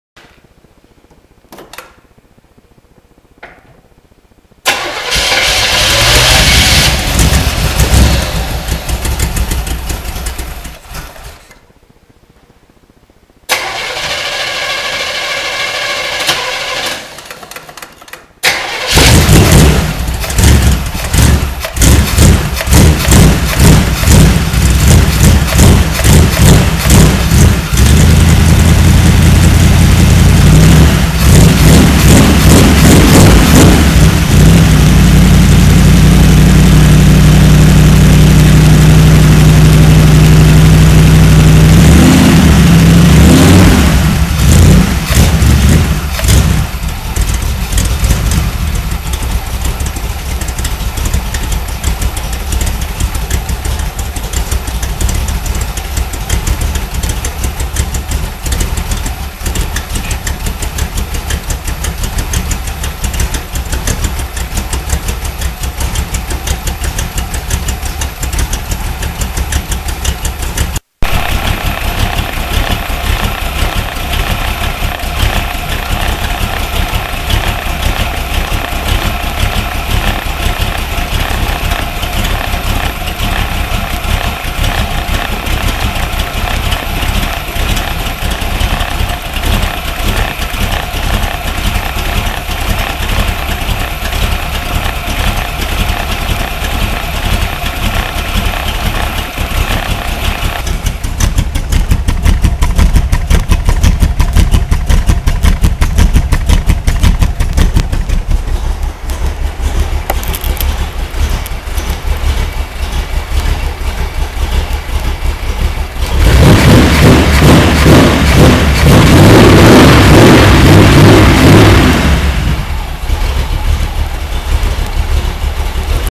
78 vette cold start under hood
cold start under the hood view,1969 327 chevy